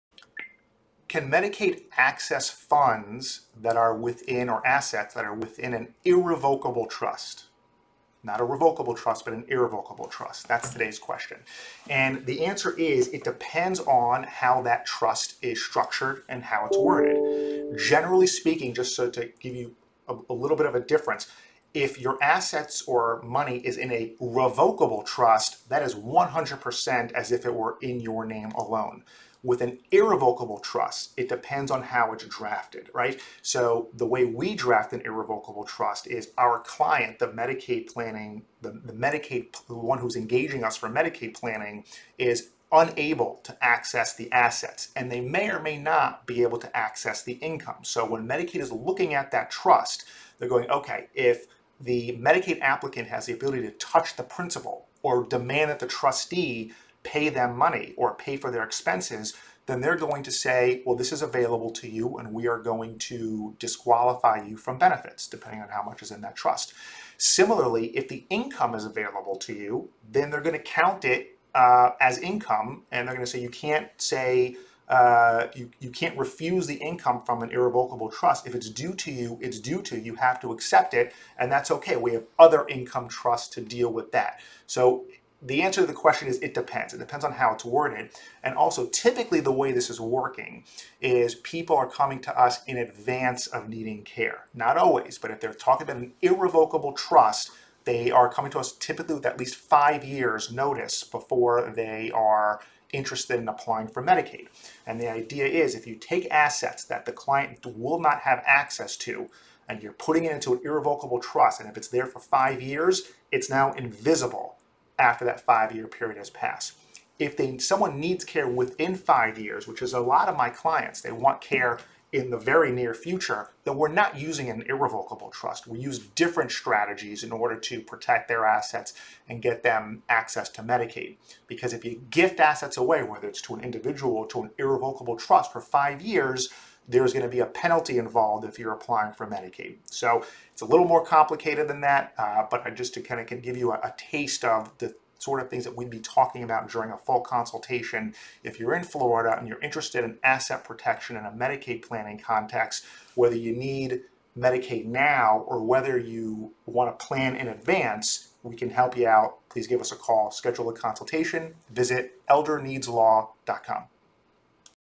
Can Medicaid Access Funds or Assets in an Irrevocable Trust?
Attorney Jason Neufeld - Founder of Elder Needs Law PLLC - Explains how and when Medicaid can access funds or assets in a trust (and how to avoid it) — Learn more about Medicaid planning 👇

🔴What Does An Elder Law Attorney Do?

Transcript:
Can Medicaid access funds that are within or assets that are within an irrevocable trust? Not a revocable trust but an irrevocable trust. That's today's question. And the answer is it depends on how that trust is structured and how its worded.

Generally speaking, just so to give you a little bit of a difference, if your assets or money is in a revocable trust that is 100%, as if it were in your name alone. With an irrevocable trust, it depends on how it's drafted. Right. So the way we draft an irrevocable trust is our client, the one who's engaging us for Medicaid planning is unable to access the assets, and they may or may not be able to access the income. So when Medicaid is looking at that trust, they're going okay, if the Medicaid applicant has the ability to touch the principle or demand that the trustee, pay them money or pay for their expenses. Then they're going to say Well, this is available to you, and we are going to disqualify you from benefits, depending on how much is in that trust. Similarly, if the income is available to you, then they're going to count it as income. And I'm going to say you can't refuse the income from an irrevocable trust. If it's due to you. It's due to you have to accept it. And that's okay. We have other income trusts to deal with that.

So the answer the question is, it depends, it depends on how it’s worded. And also typically the way this is working, is people are coming to us in advance of needing care, not always. But if they're talking about an irrevocable trust, they are coming to us typically with at least five years notice before they are interested in applying for Medicaid. And the idea is if you take assets that the client will not have access to, and you're putting it into an irrevocable trust. And if it's there for five years, it's now invisible. After that five year period has passed.

If then someone needs care within five years, which is a lot of my clients, they want care in the very near future, that we're not using an irrevocable trust, we use different strategies in order to protect their assets and get them access to Medicaid. Because if you gift assets away whether it's to an individual or to an irrevocable trust for five years, there's going to be a penalty involved if you're applying for Medicaid. So it's a little more complicated than that. But just to kind of give you a taste of the sort of things that we'll be talking about during a full consultation.

If you're in Florida and you're interested in asset protection in a Medicaid planning context, whether you need Medicaid now or whether you want to plan in advance we can help you out please give us a call schedule a consultation visit elderneedslaw.com